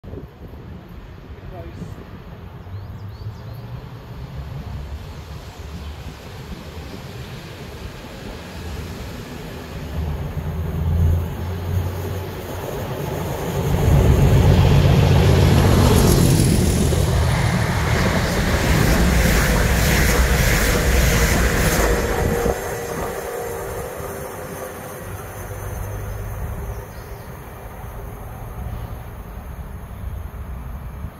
Angle 2 - 37884 seen again. Close call!
37884
769935

Train 5Q23 Long Marston to Doncaster Works Wagn Shops
Operated By ROG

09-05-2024